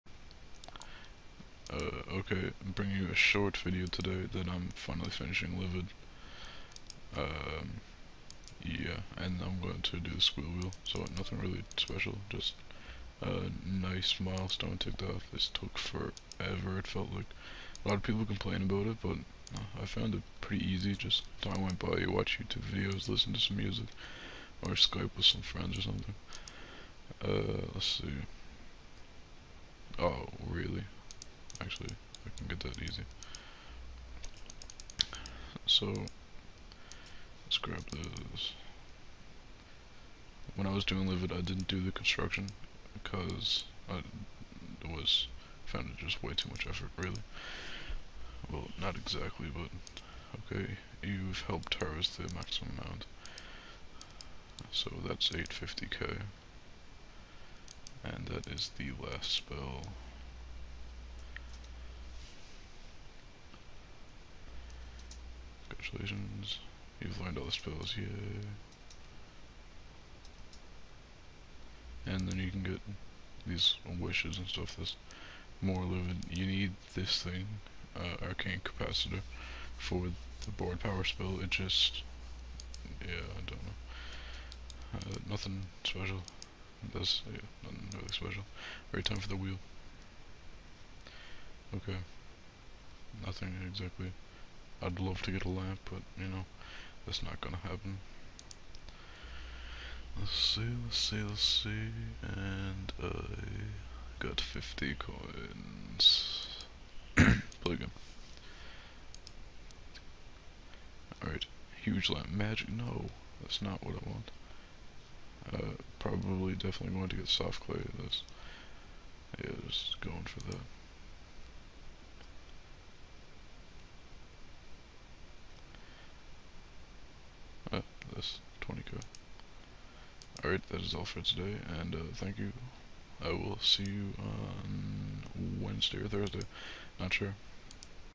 Livid Completed
Yayy livid is DONE!
After many hours and 2, maybe 3, farm levels gained, livid is finally completed. I'm not sure if there is audio in this video as the first big blob of text says. I was reviewing it seeing if anything needed to be placed anywhere and it just went blank. Closed out of camtasia, reopened, nothing. :( Oh well, HOPEFULLY it worked for next rtcc. I'll have to try again.
On a side note, I can record skype :)